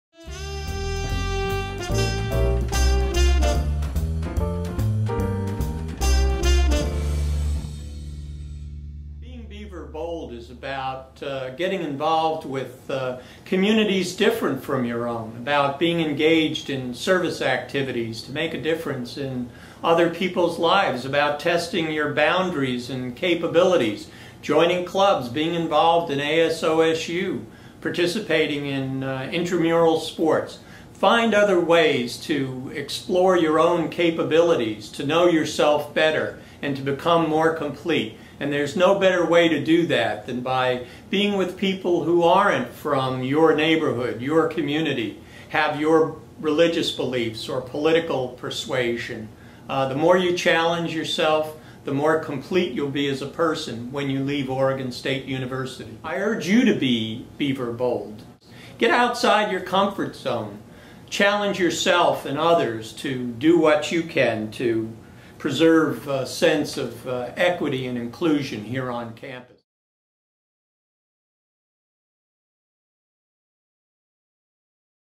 President Ed Ray
Oregon State University President, Ed Ray explains what it means to beBEAVERBOLD.